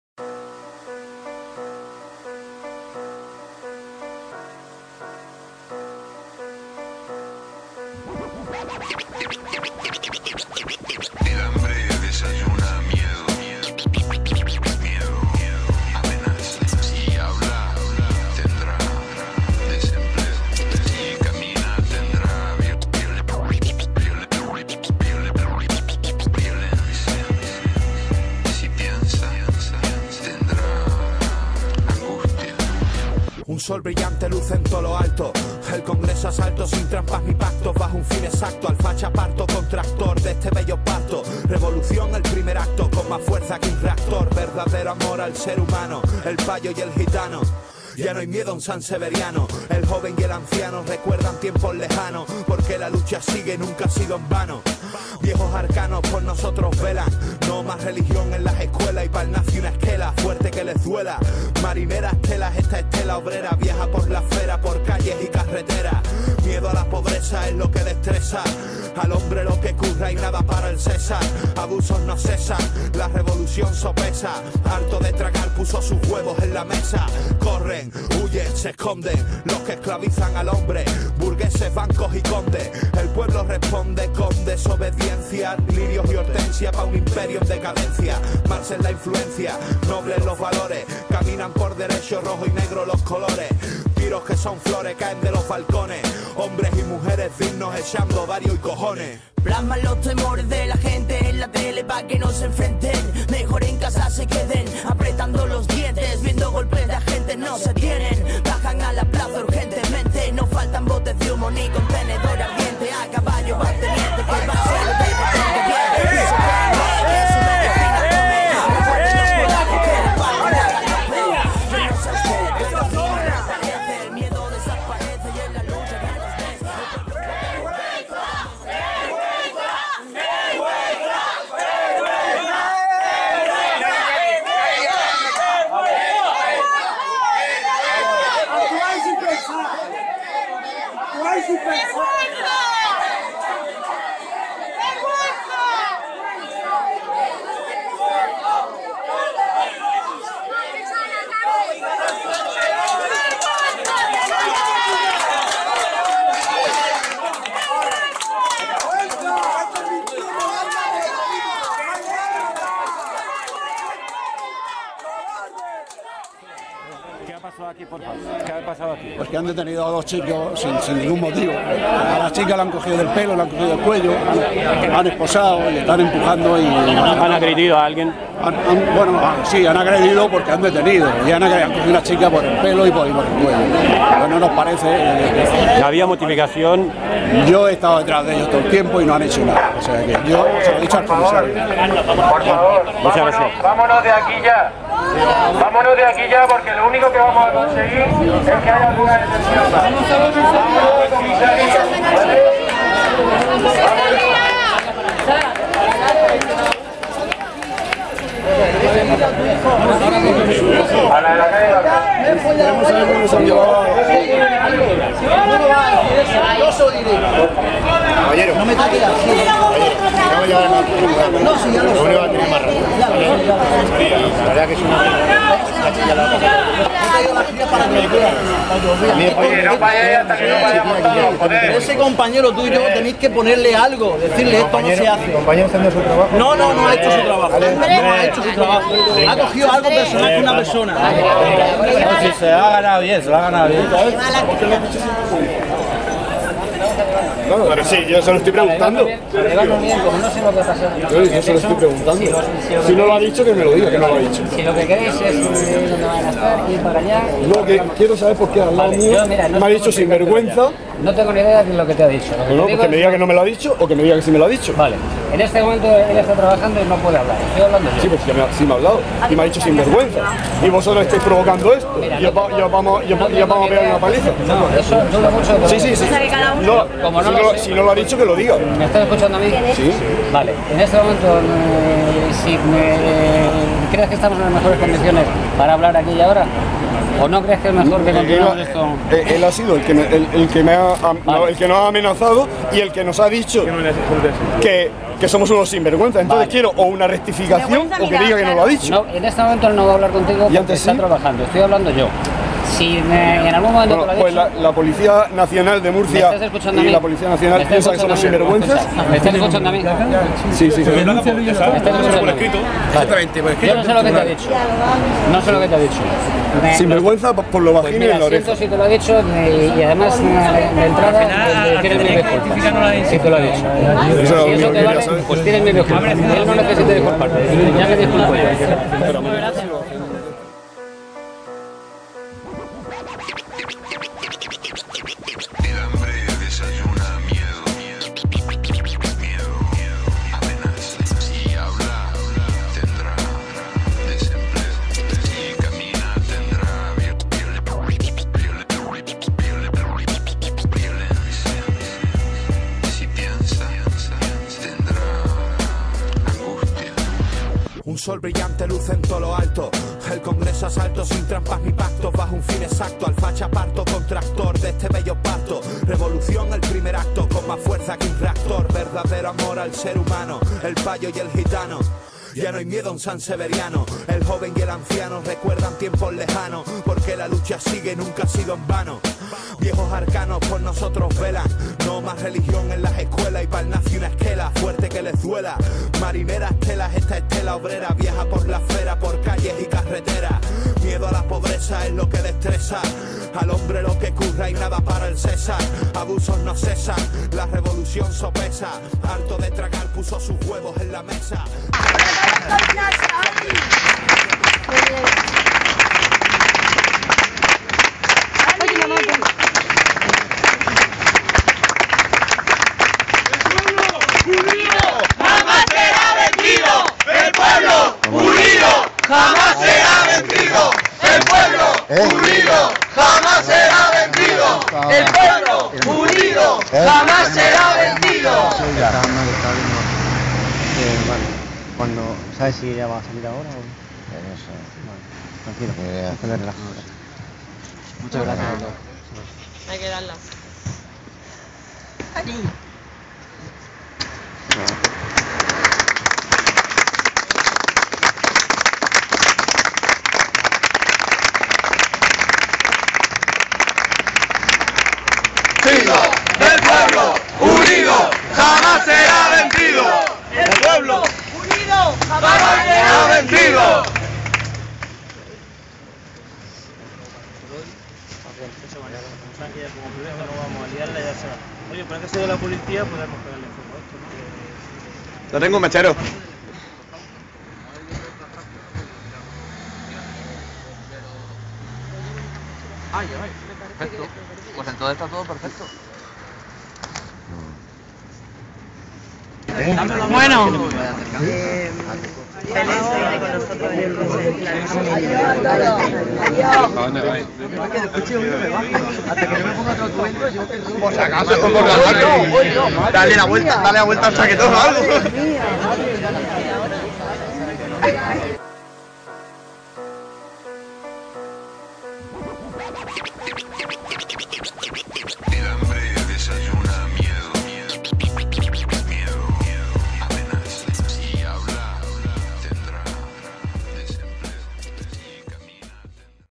2 detenidxs y carga policial en la visita de Rajoy a Murcia 14D (VI)
2 detenidxs y carga policial en la visita de Rajoy a Murcia 14D (VI)
Murcia - Spain
14/12/2013
Está pasando...